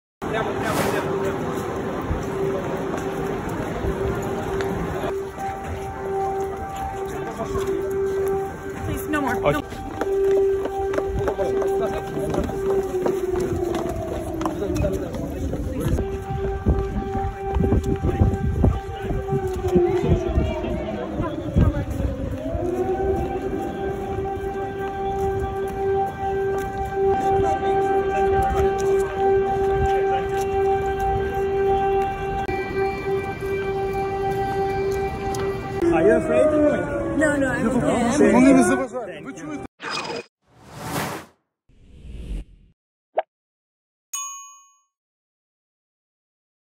📣💥АНДЖЕЛІНА ДЖОЛІ ПІД ЧАС ПОВІТРЯНОЇ ТРИВОГИ У ЛЬВОВІ | ANGELINA JOLIE DURING THE AIR ALARM IN LVIV
Коли Анджеліна Джолі була на вокзалі у Львові, почалася повітряна тривога. Зірка разом з іншими людьми бігла, куди вказали волонтери. На запитання: "Чи вам страшно?", відповіла: "Ні, ні, я в порядку".

Ukraine.Media
Lviv.Media